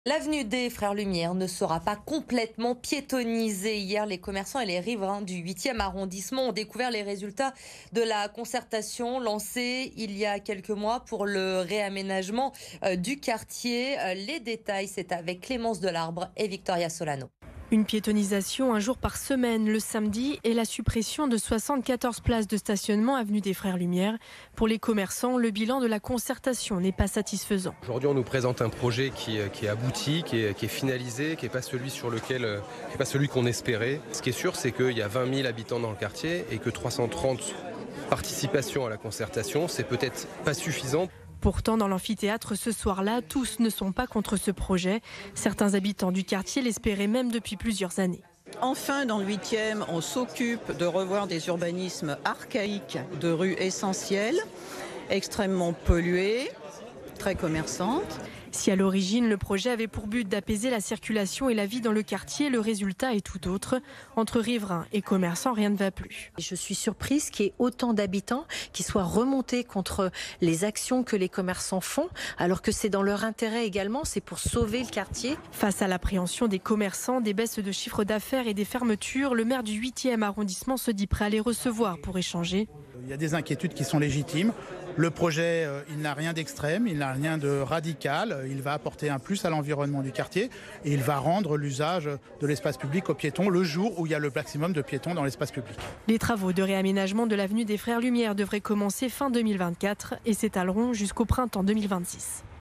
Lyon 8e : piétonnisation uniquement les samedis pour l'avenue des Frères Lumières
L'avenue des Frères Lumières ne sera pas complètement piétonne. Les habitants du 8e arrondissement ont découvert lors d'une réunion publique les résultats du projet définitif d'apaisement de cette grande artère de Lyon.